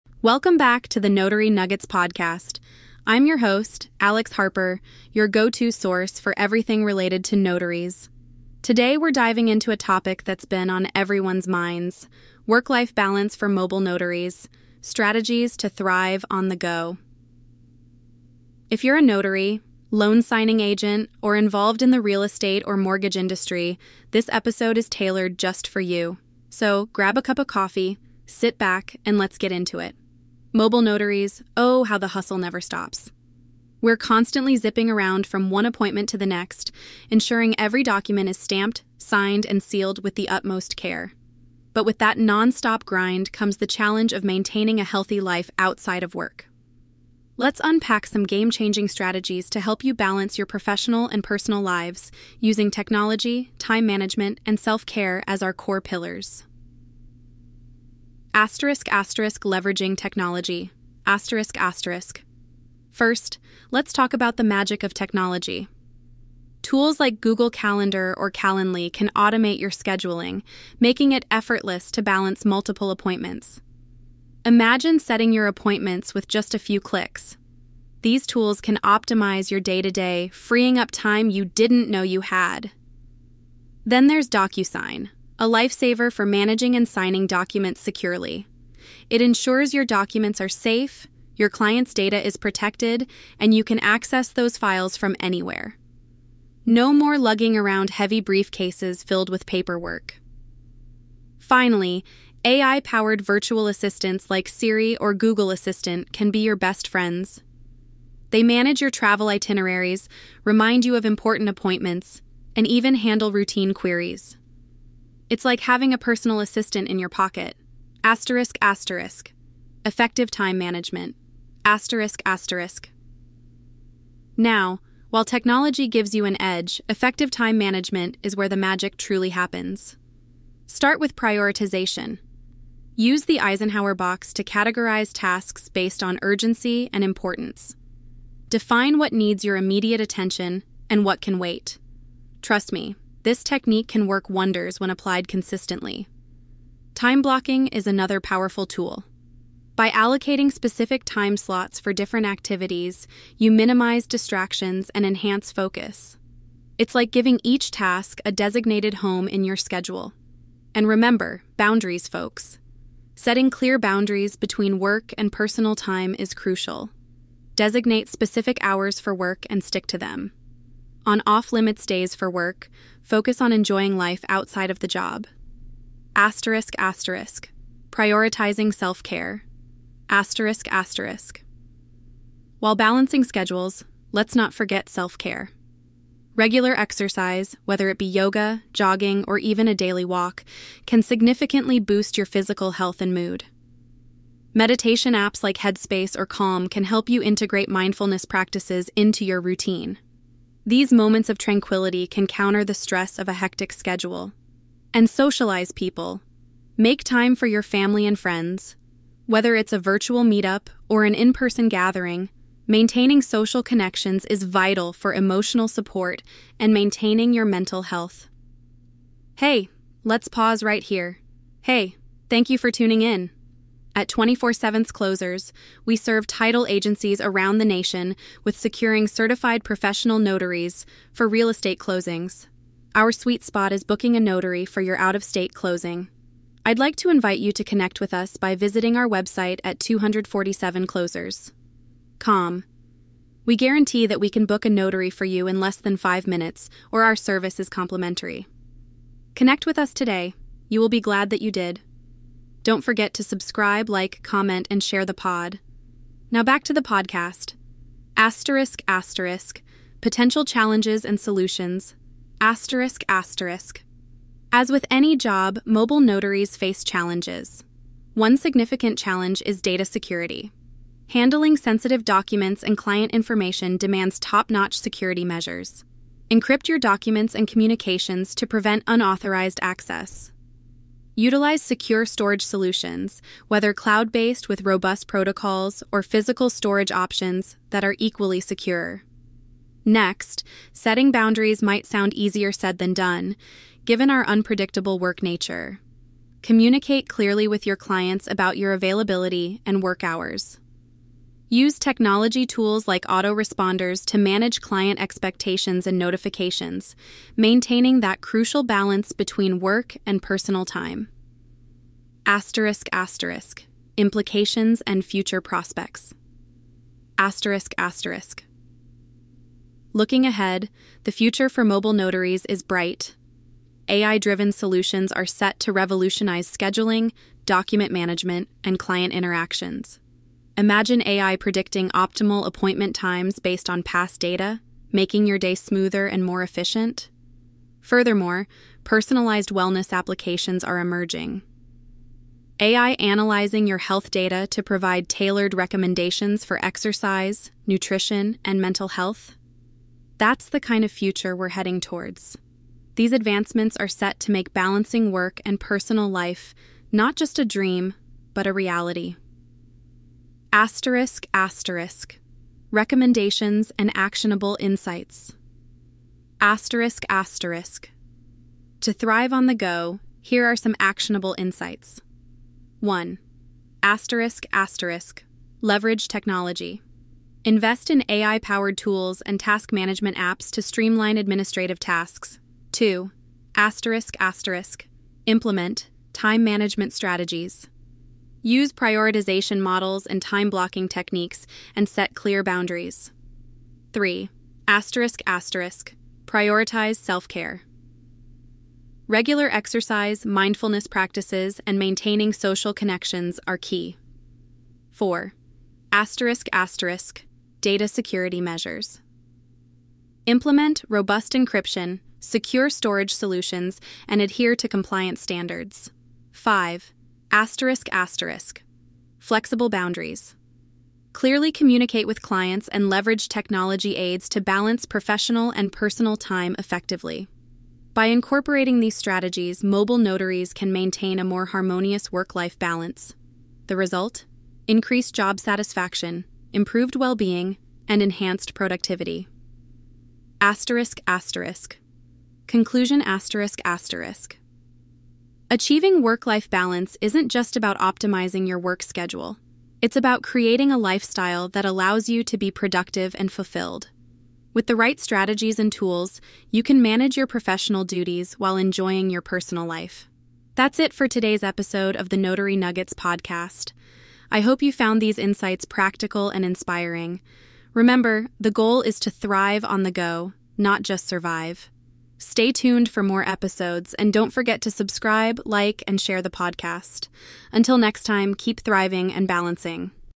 Mobile Notaries: Thriving Work-Life On the Move
In this episode of The Notary Nuggets Podcast, we dive into the bustling world of mobile notaries. Discover how these pros manage a thriving business while enjoying the flexibility of life on the move. Expert guests share tips for balancing work and personal life, efficiency hacks, and stories from the road. Tune in for your blueprint to notary success on the go!